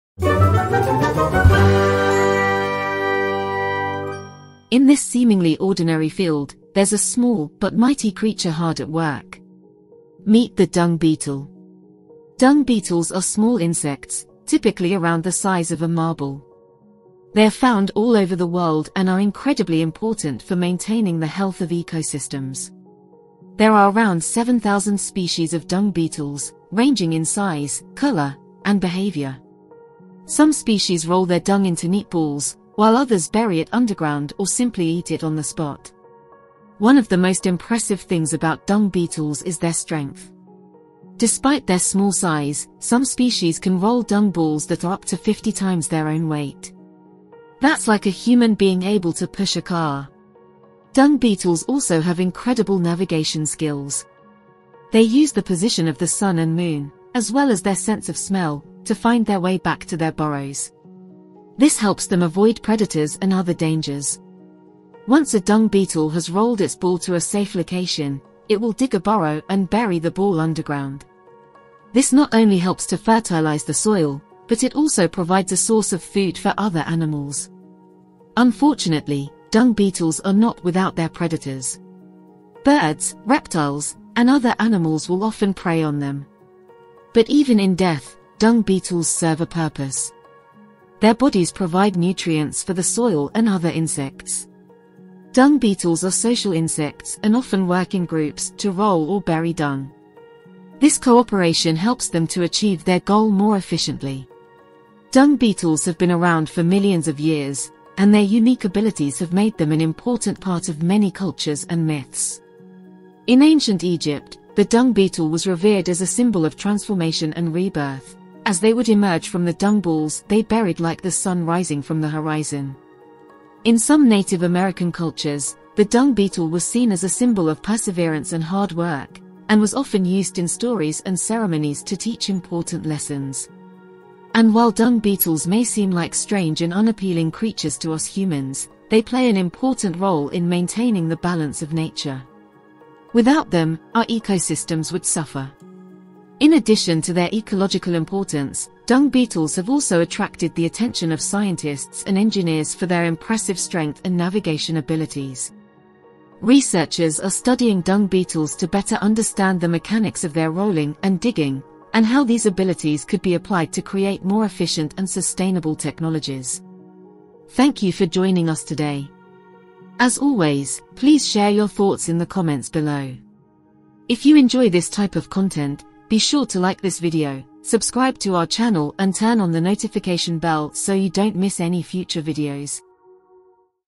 Dung Beetles | Super STRENGTH & Navigation Skills!
Did you know that dung beetles are actually super strong? That's how they are able to roll balls of dung much larger in size than they are. They also have incredible sense of direction.